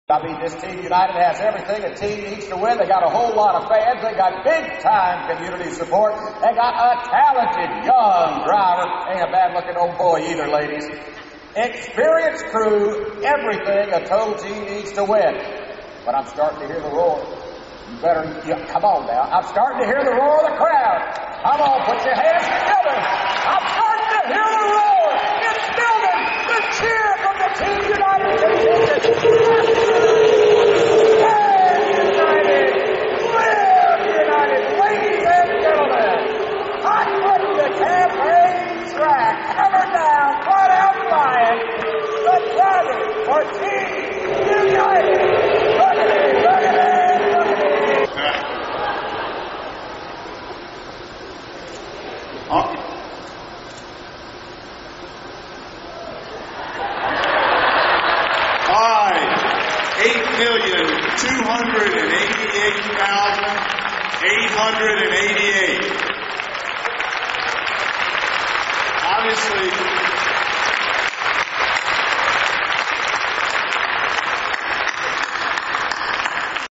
United Way Kick-off
See video of the unvailing of the 2008 campaign theme and goal.